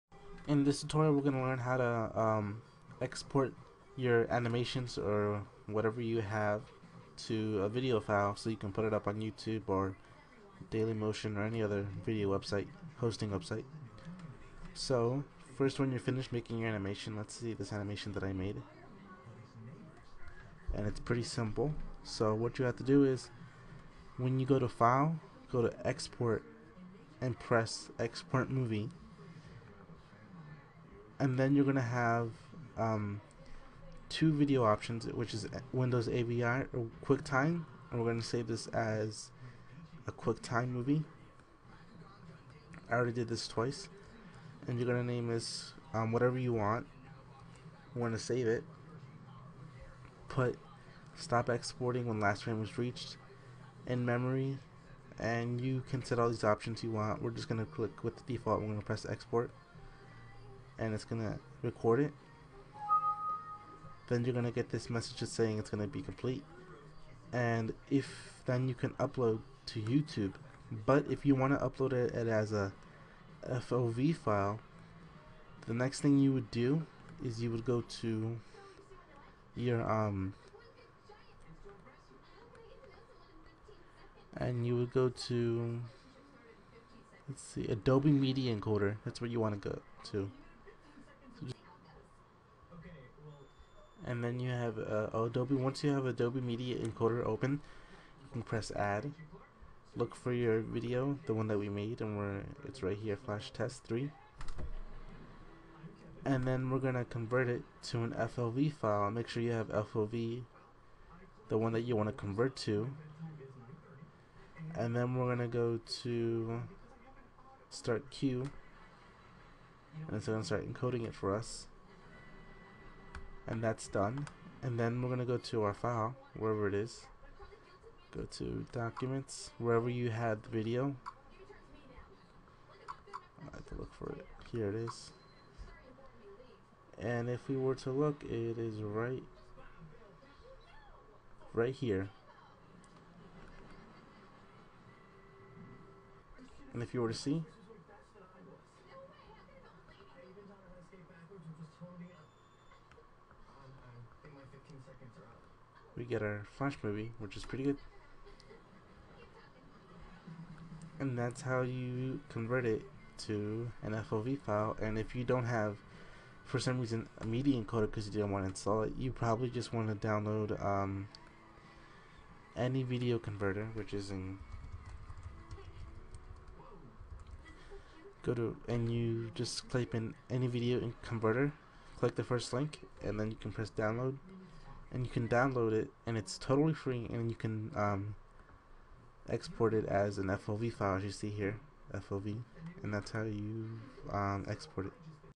Flash CS4 Animation Tutorial(Export Animations 09)
Learn  how to export your animations into video files and upload them to video hosting websites such as youtube